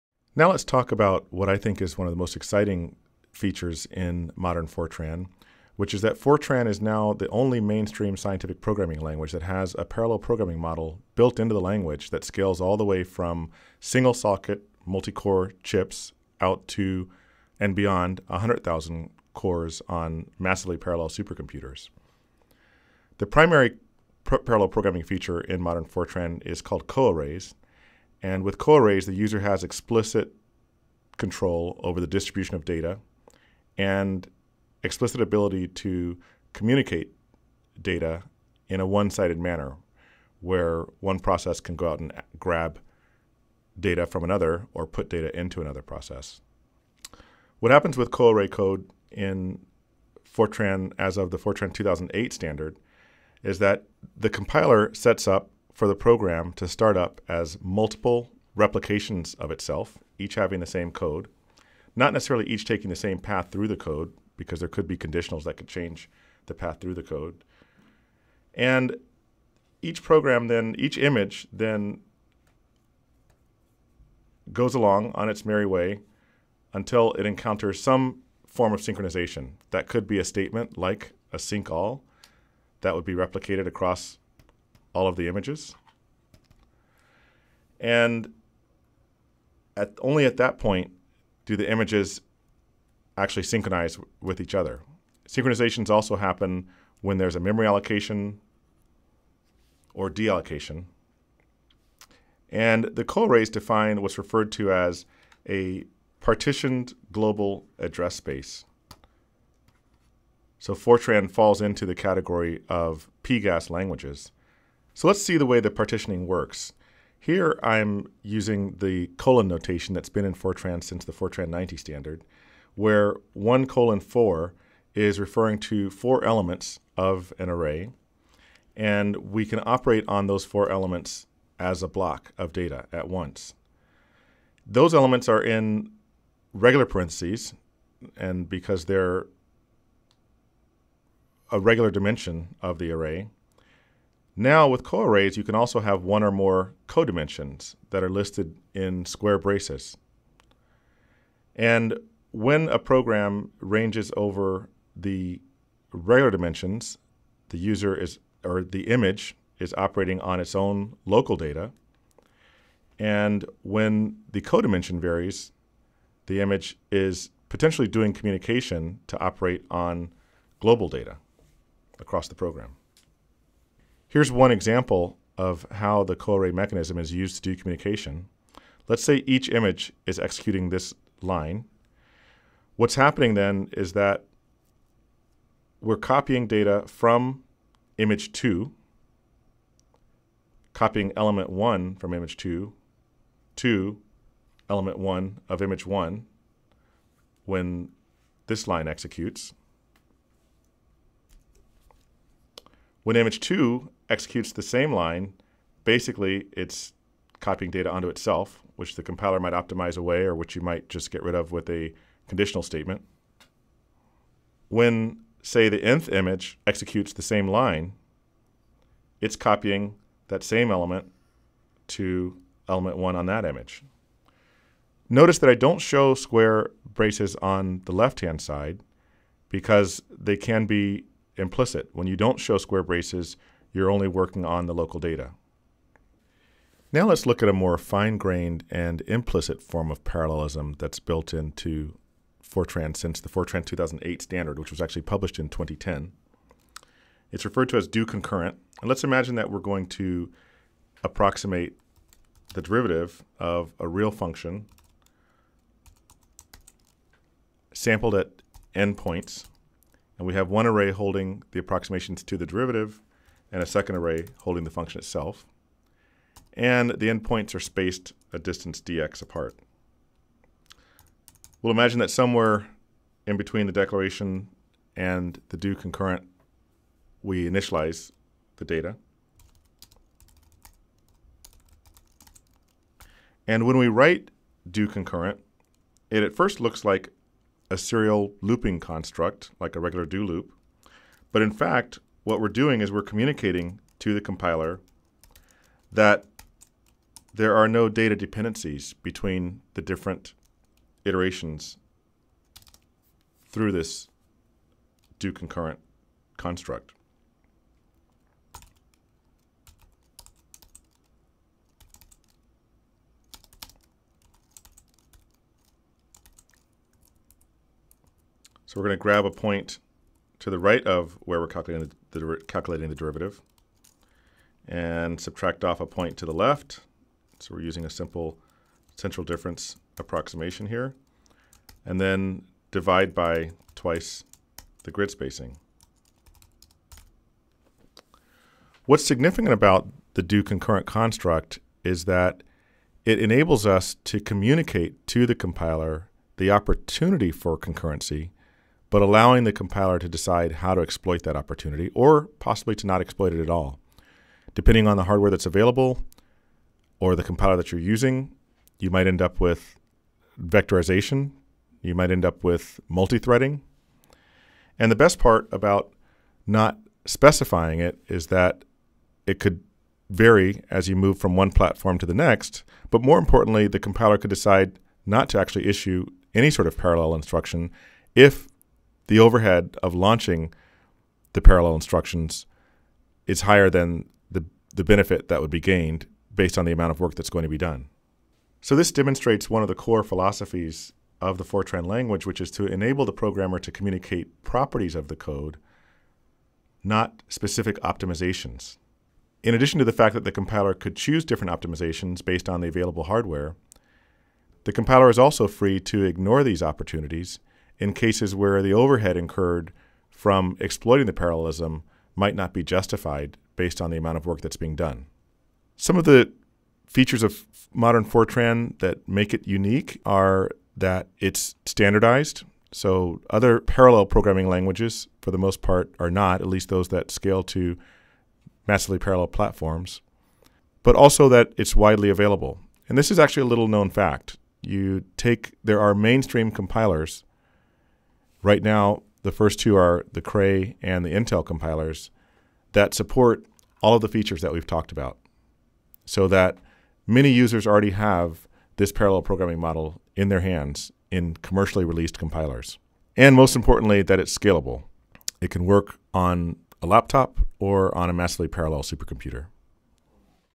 Parallel Programming in Modern Fortran
Introducing the coarray parallel programming features of Fortran 2008 and beyond.